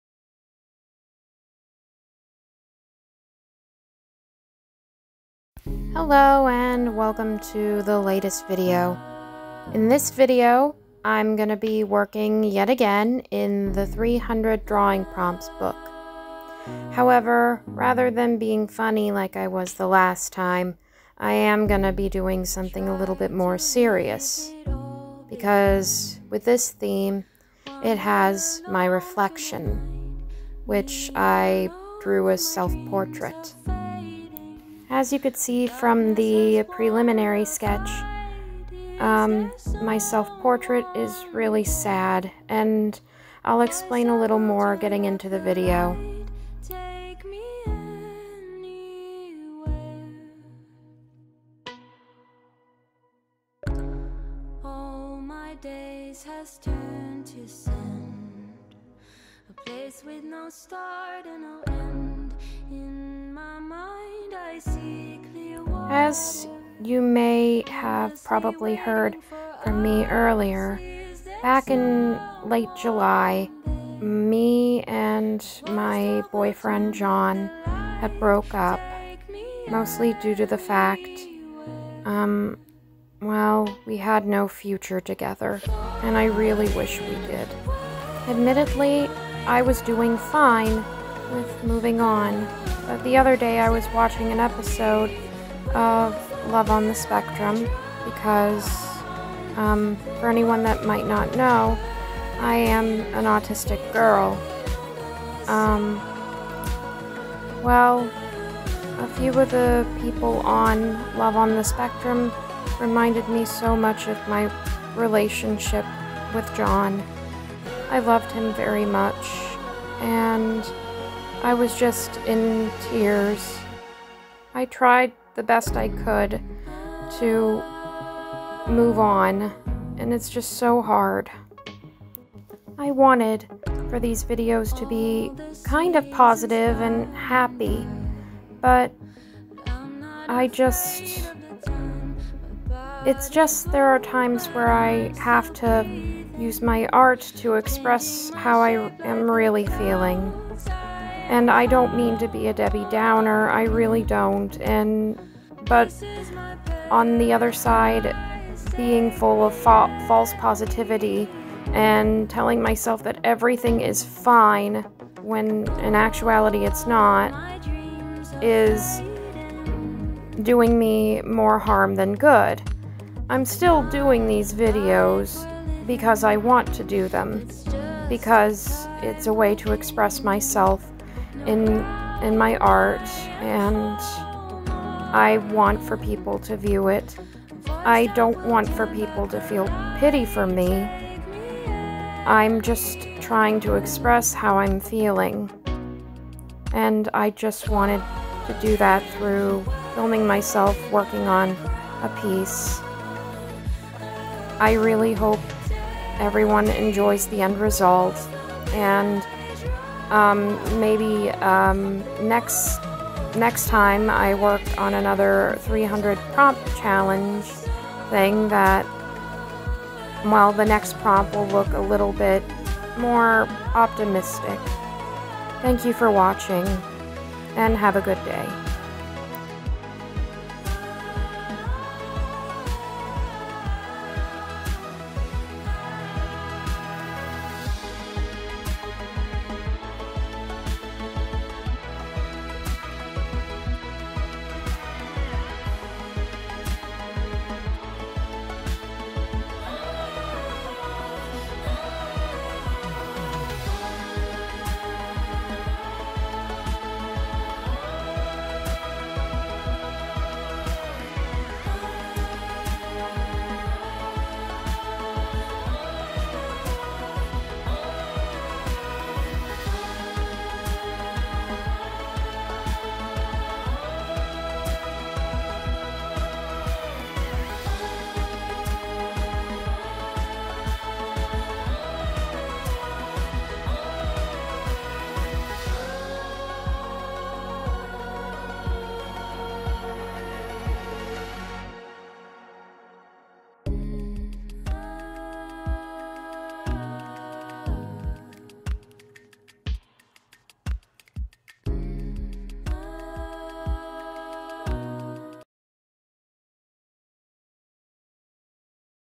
300 Art Prompt - My Reflection or "I Am Nothing Without You"
Edited with FilmoraGo, in this video, I worked on another prompt from the book, 300 Art Prompts with the prompt being my own reflection. I don't like drawing self portraits, but for this, I'll make an exception.

Materials
• Canson Mixed Media Paper
• Sakura Micron PN
•Pilot Brush Pen
•Faber Castell SB
• Ohuhu Alcohol Markers
• COPIC Markers
•Artify Alcohol Marker
• STA Alcohol Marker
• Style File Marker
• Koi Watercolor Brush Pens in Light and Dark Green